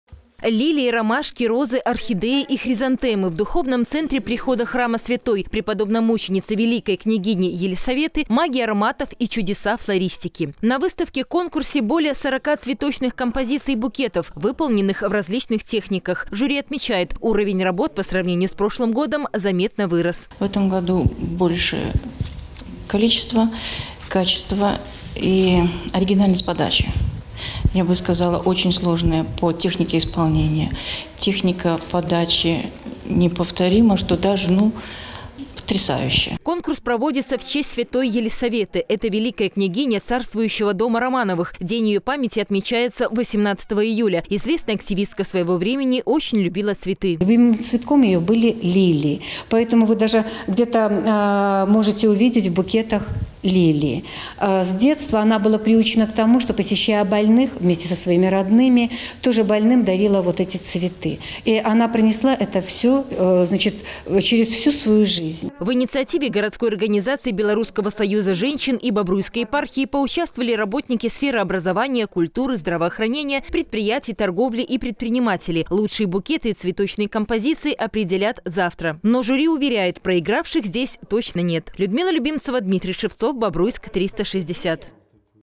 ЦВЕТЫ ДЛЯ ВЕЛИКОЙ КНЯГИНИ
В Духовном центре прихода храма святой преподобномученицы Елисаветы проходит выставка-конкурс флористических композиций и букетов.